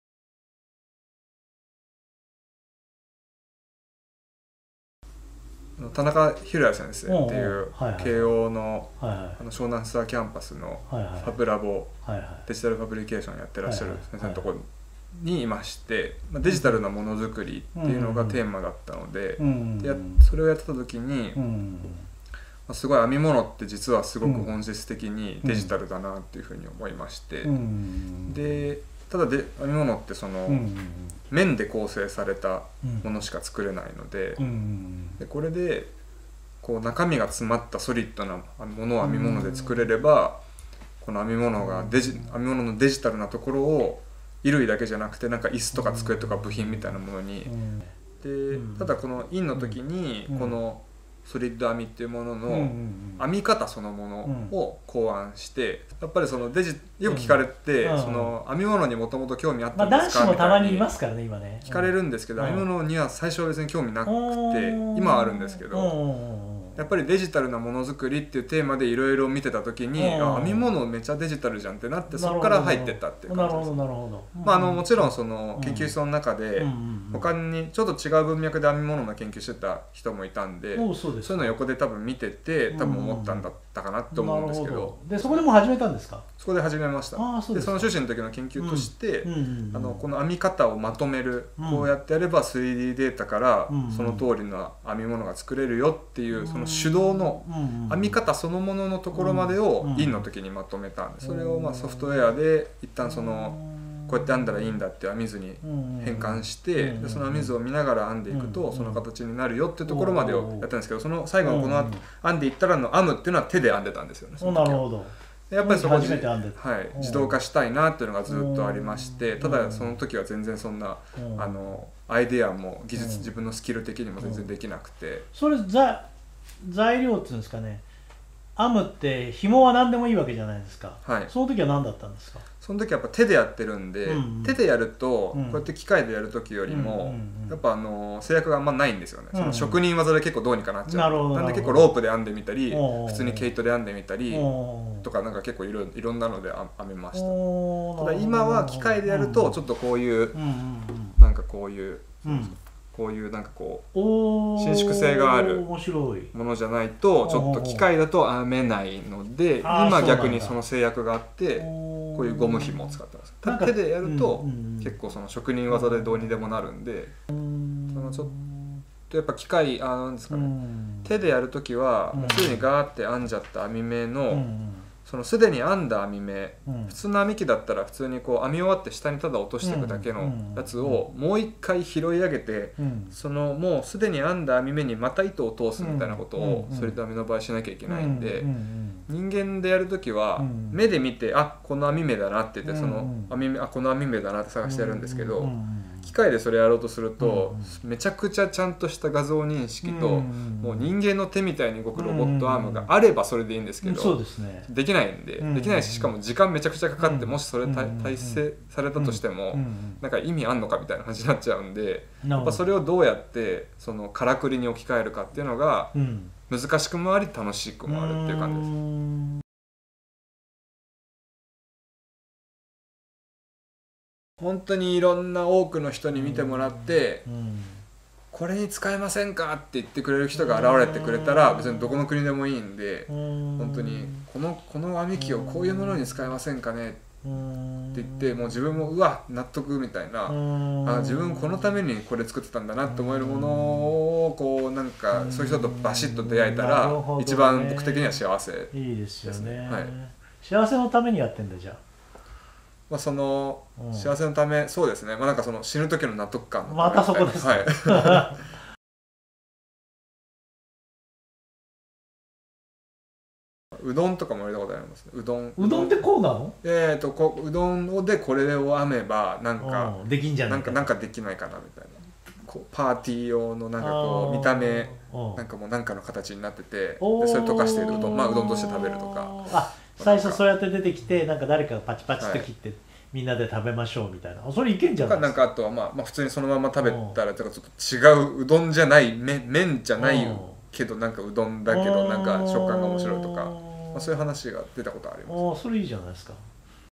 Are Knitting Machines the Next Frontier in 3D Printing? | InnoUvator Interviews
Yuichi Hirose has come up with a novel approach to 3D printing. By utilizing the techniques found in knitting, he has been able to create structures that are solid and could potentially be the next step in 3D printing innovation.

About Yuichi Hirose

After finishing his master’s degree in digital fabrication at the Hiroya Tanaka Laboratory of Keio University, he worked for Roland DG as a mechanical designer on 3D milling machines. Now, he has left the company and develops the 'solid knitting machine' which automates 'solid knitting,' a novel method he invented to knit dense objects from 3D data.

Credits:

CG Knitting demonstration
GROZ-BECKERT®